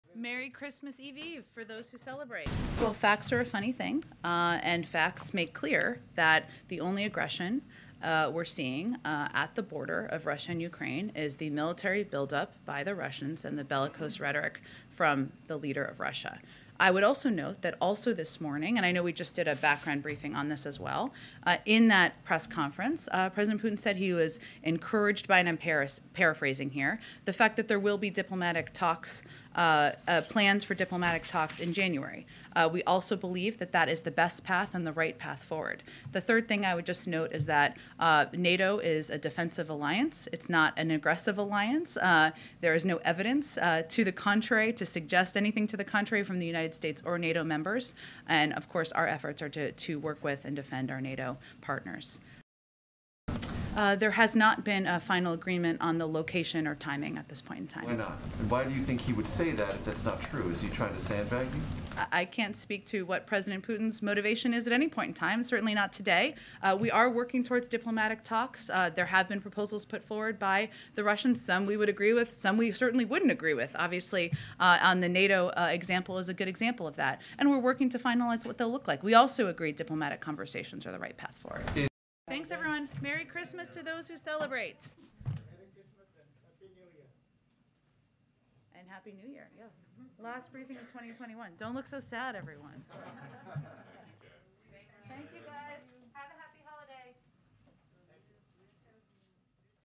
White House says no agreement on new Biden-Putin talks
U.S. President Joe Biden has yet to agree to a time and place of fresh talks with Russian President Vladimir Putin, White House spokesperson Jen Psaki said on Thursday.